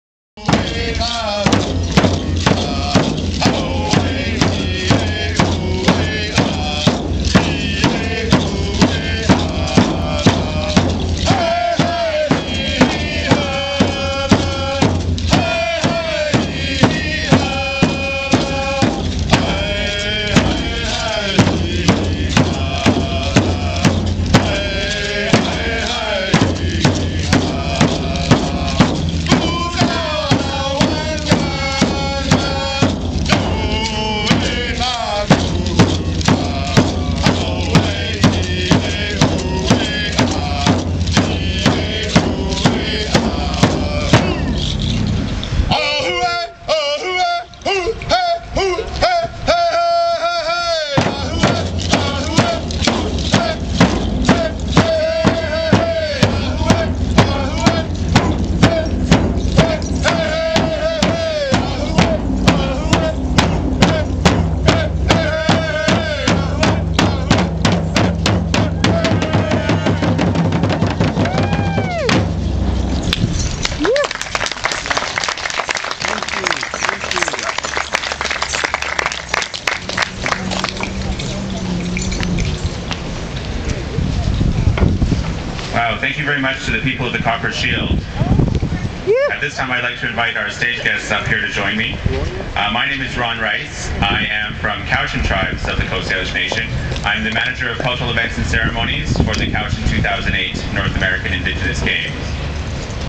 Spirit Pole on the road 037.mov
Spirit Pole cultural dancers, Vancouver British Columbia